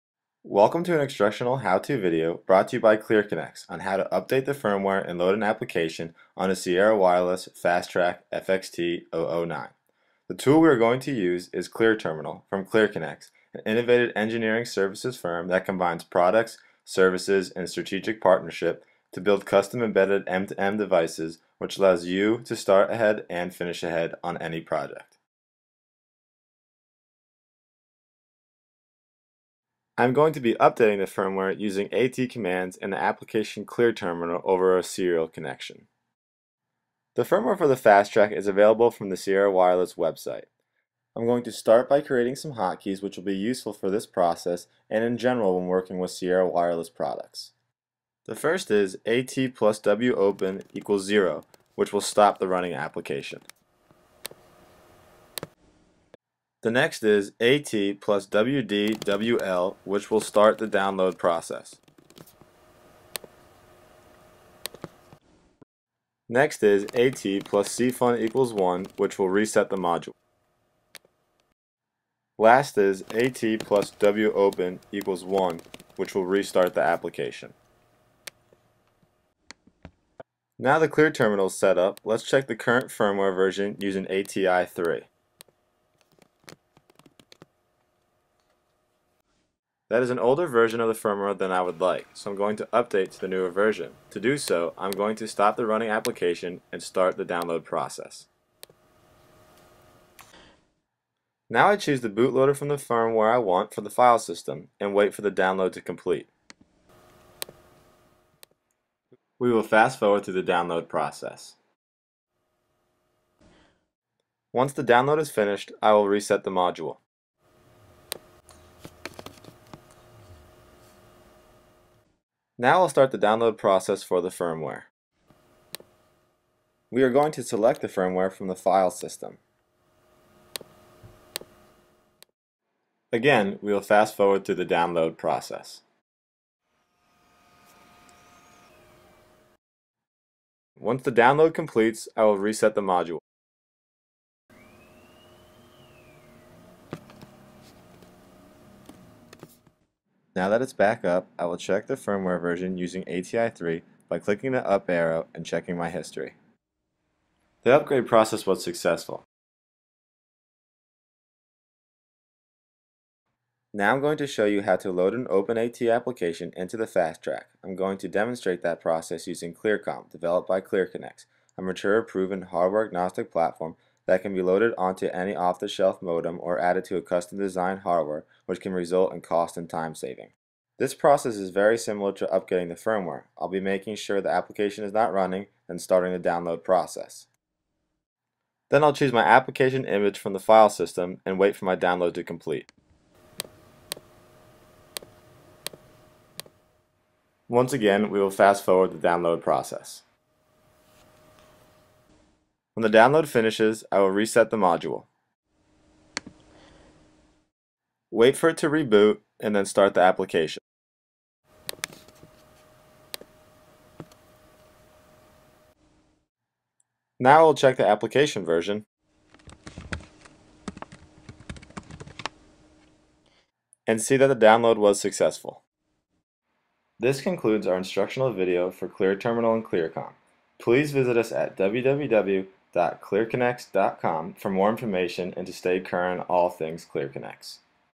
How to use ClearTerminal to Update Firmware on the Sierra Wireless Fastrack Xtend FXT009
Starting Ahead and Finishing Ahead is made even easier using ClearConnex's how-to video series. This instructional video illustrates how to update firmware and load the ClearComm application on the Sierra Wireless FXT009 using ClearTerminal.